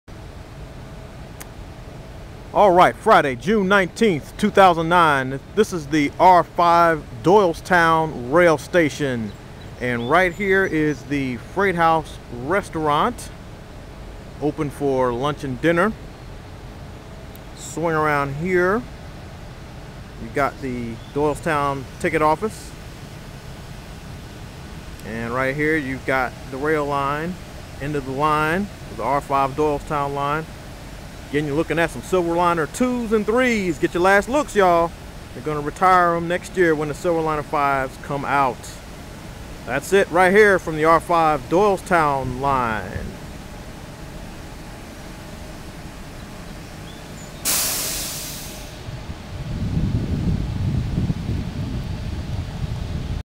R5 Doylestown at Doylestown and Freight House
We see the Freight House Restaurant and the R5 Doylestown at Doylestown Station.